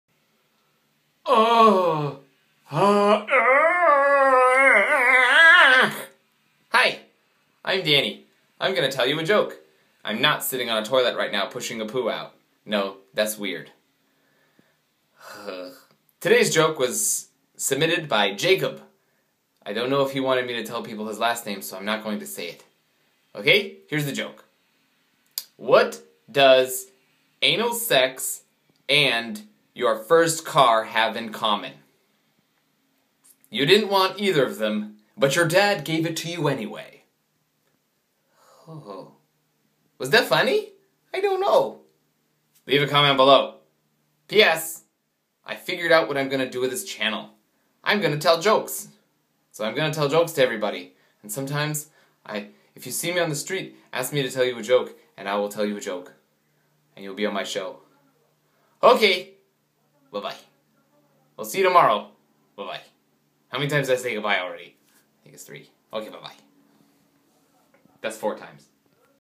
Anal sex joke
What does anal sex and your first car have in-common?
You didn't really want it, but your dad gave it to you anyway.